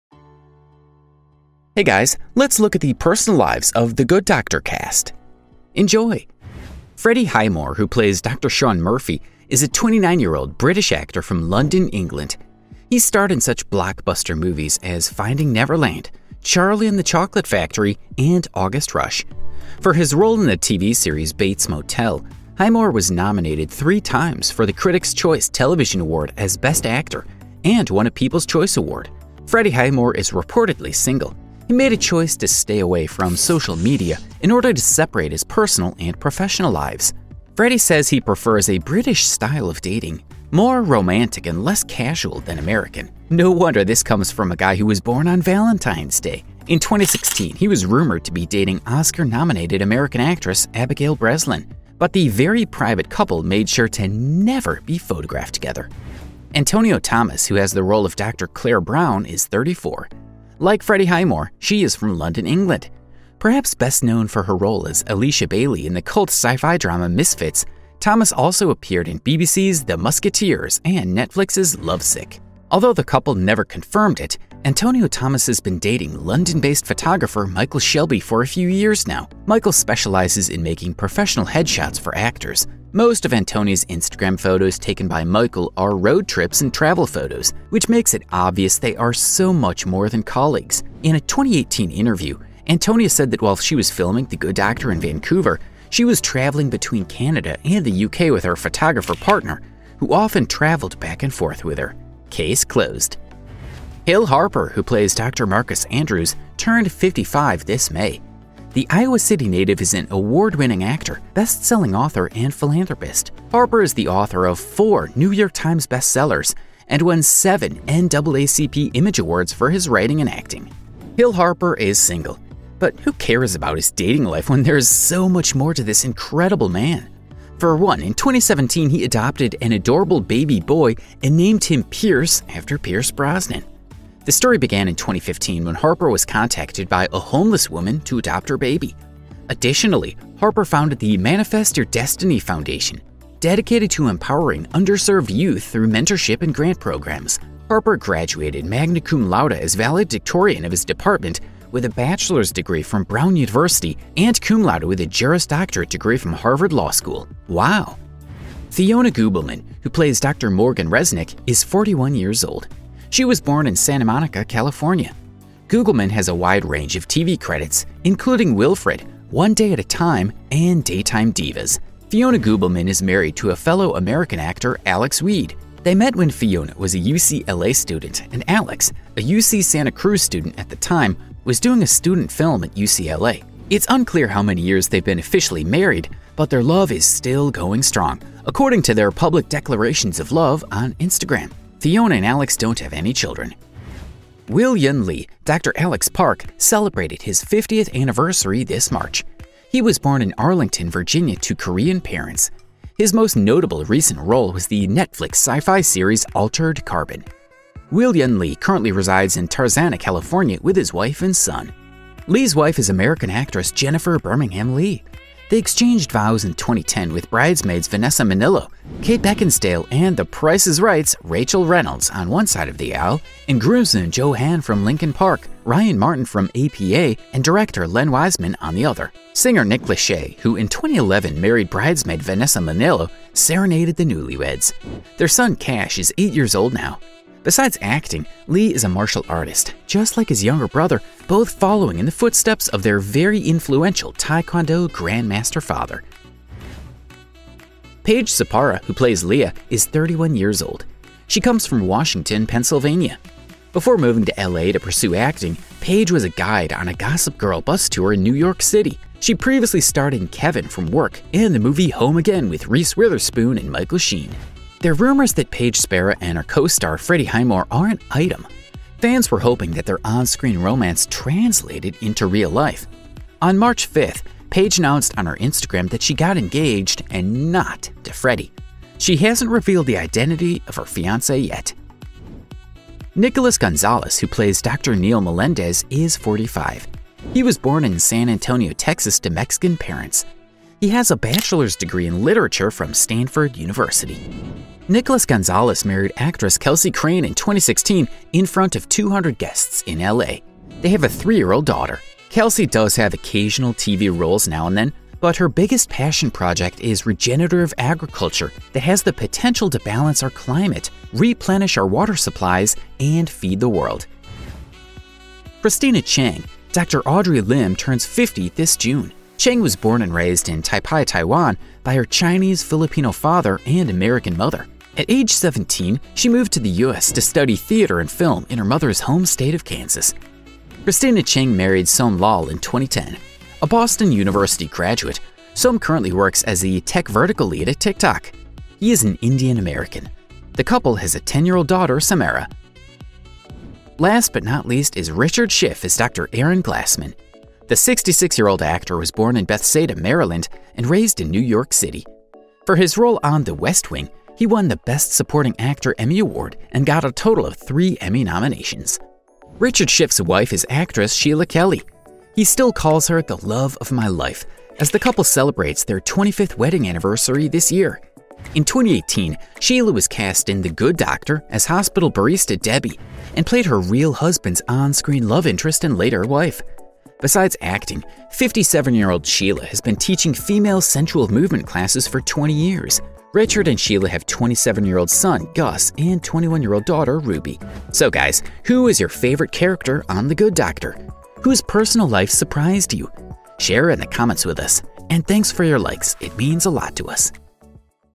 THE GOOD DOCTOR Season 5 Cast Real-Life Partners
Hey, guys! As you wait for Season 5 release of The Good Doctor, let’s take a peek into the personal, family and dating lives of its beautiful international cast.
In this video you’ll learn why Freddie Highmore is not on any social media. You’ll find out if rumors of Freddie Highmore & Paige Spara dating in real life are true and discover a star-studded guest list of Will Yun Lee’s wedding. Enjoy!
Who is YOUR favorite cast member? Share in the comments!